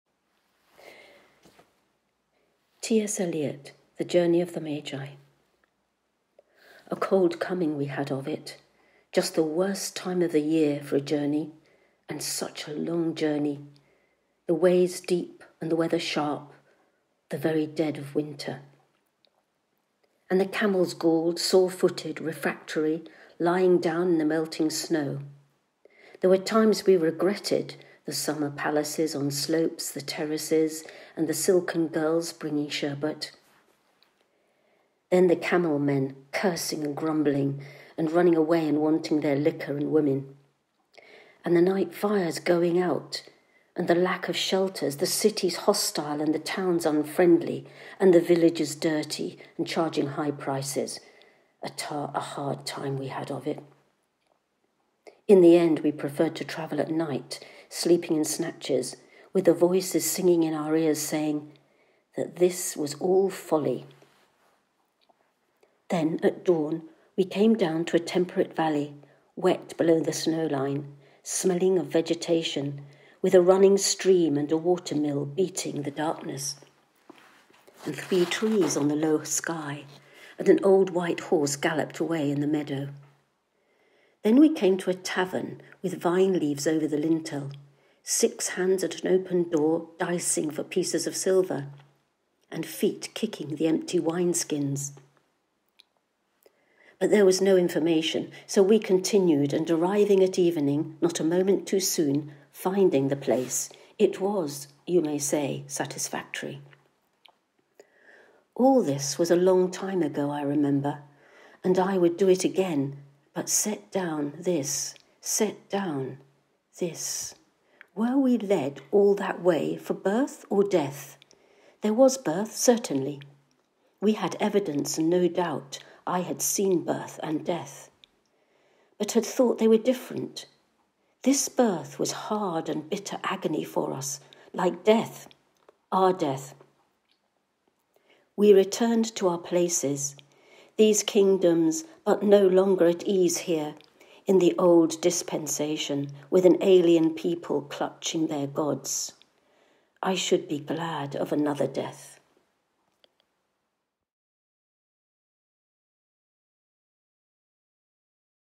Epiphany Poems #1: Journey of the Magi T.S. Eliot
Poem read by Anne Lattul from St Martin-in-the-fields
Video: Fr. Richard Carter Saturday Contemplation